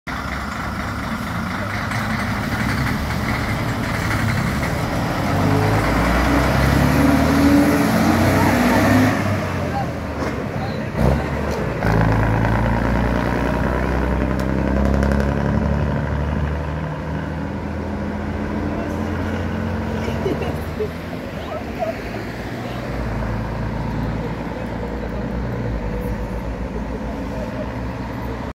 Ikarus 620 118MPK Kraków Linia W4 13.07.2024r. - Piekary Śląskie:odjazd w kierunku Starych Tarnowic
Przejażdżka zaliczona to na koniec jeszcze tylko odjazd. Pozdrawiam osoby machające z tyłu autobusu.